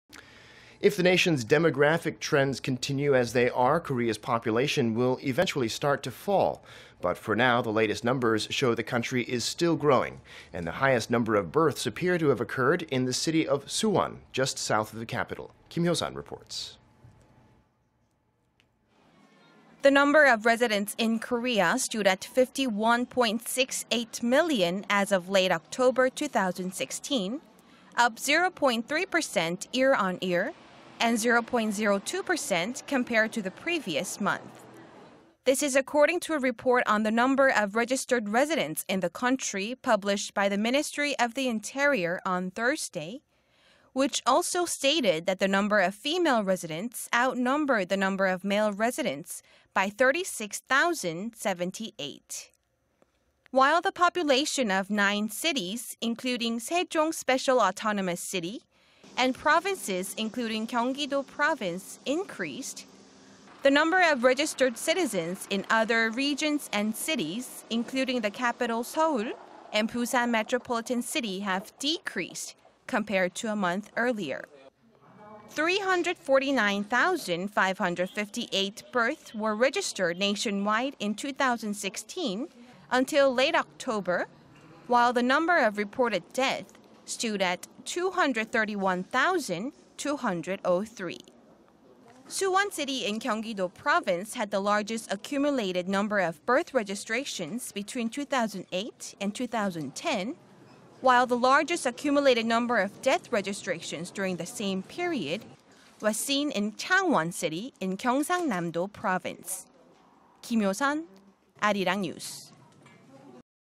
Number of Korean residents stands at 51.68 million
전국 주민등록인구 5천168만… 수원시, 출생신고 으뜸
  If the nation's demographic trends continue as they are, Korea's population will eventually start to fall.
But for now, the latest numbers show the country is still growing.
And the highest number of births appear to have occured in city of Suwon, just south of the capital.
Kim Hyo-sun reports.   The number of residents in Korea stood at 51.68 million as of late October, 2016,... up 0.3 percent year-on-year and 0.02 percent compared to the previous month.
  This is according to a report on the number of registered residents in the country published by the Ministry of the Interior on Thursday... which also stated that the number of female residents outnumbered the number of male residents by 36,078.
While the population of nine cities including Sejong Special Autonomous City, and provinces including Gyeonggi-do Province, increased,... the number of registered citizens in other regions and cities, including the capital Seoul and Busan Metropolitan City have decreased compared to a month earlier.
  349,558 births were registered nationwide in 2016 until late October... while the number of reported deaths stood at 231,203.
  Suwon City in Gyeonggi-do Province had the largest accumulated number of birth registrations between 2008 and 2010,... while the largest accumulated number of death registrations during the same period was seen in Changwon City in Gyeongsangnam-do Province.